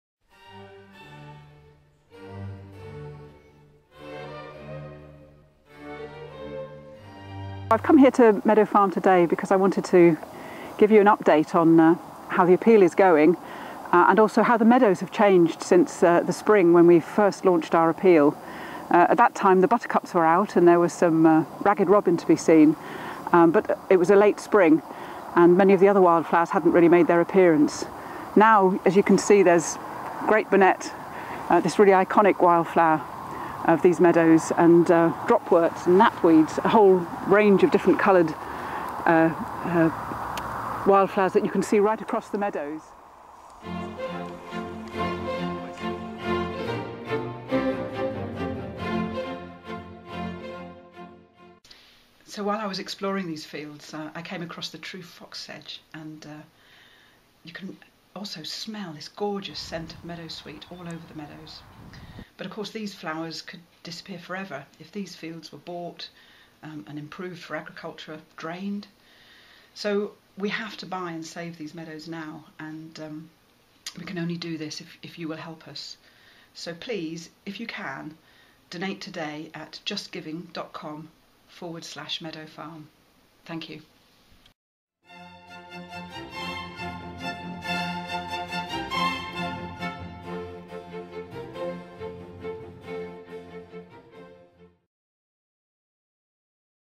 Save Meadow Farm
Meadow Farm is a rare and precious remnant of a wonderfully diverse habitat that once covered swathes of river valleys across the UK. Today, due to the pressures of modern farming and development, just 1,500 hectares of this type of habitat remain in England.

We have a once-only opportunity to save this rare habitat from destruction and use it to generate new wildlife habitats beyond its boundaries. But we have to raise the funds before the end of July.